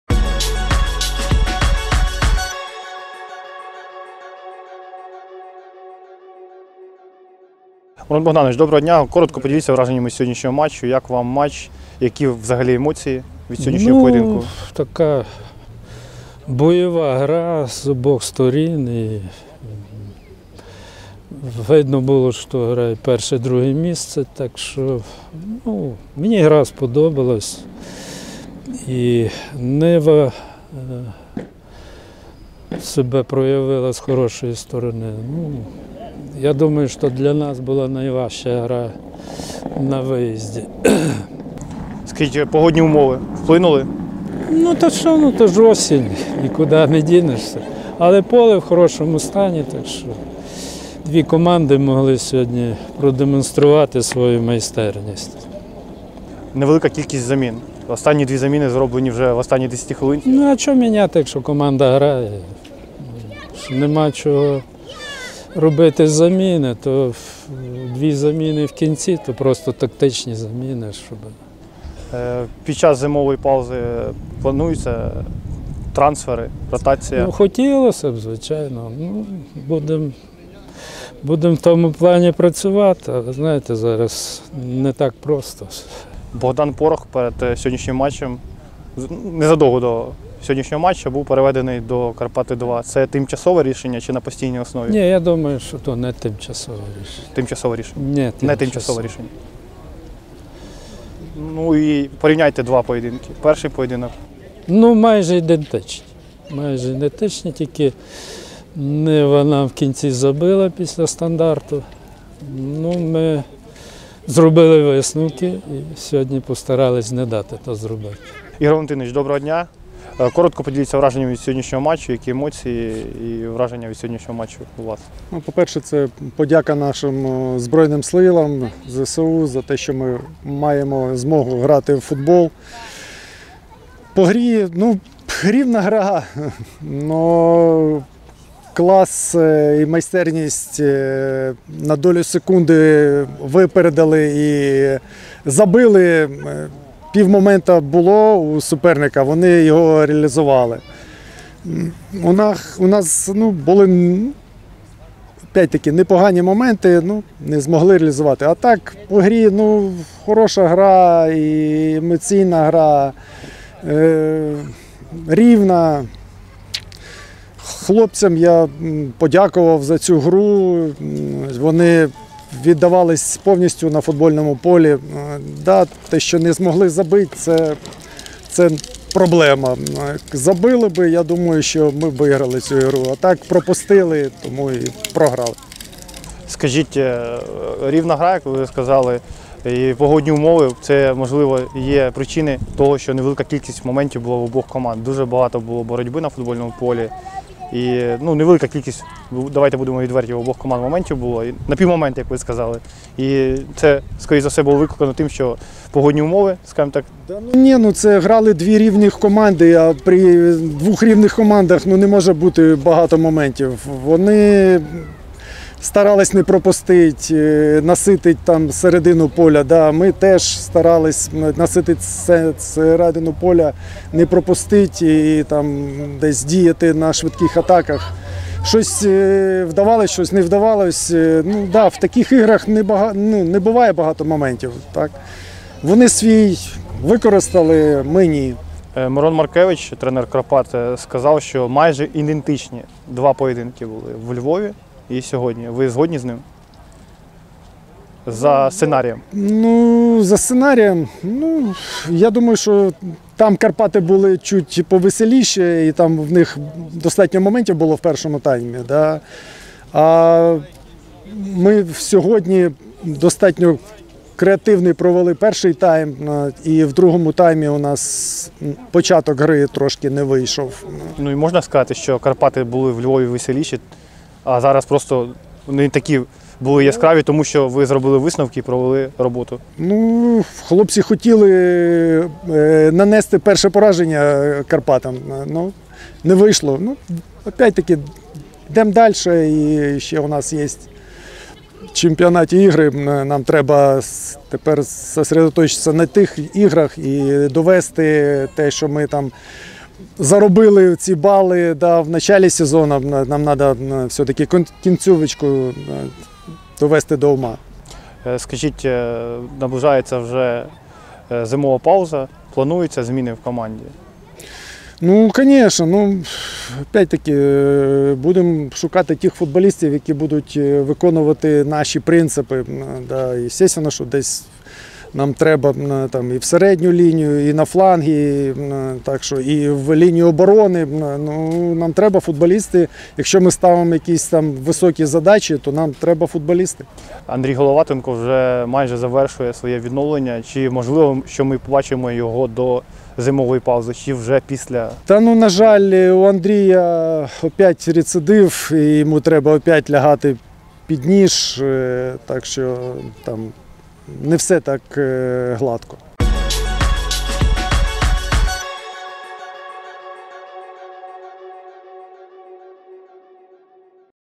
Нива (Бузова) – Карпати. ПІСЛЯМАТЧЕВІ КОМЕНТАРІ / Перша ліга ПФЛ
У матчі тринадцятого туру групи «А» Першої ліги ПФЛ сезону 2023/24 «Нива» Бузова мінімально програла «Карпатам». Поєдинок завершився з рахунком 0:1. До вашої уваги післяматчеві коментарі тренерів. Наставник «Карпат» Мирон Маркевич та тренер «Ниви» Бузової Ігор Жабченко поділились своїми враженнями.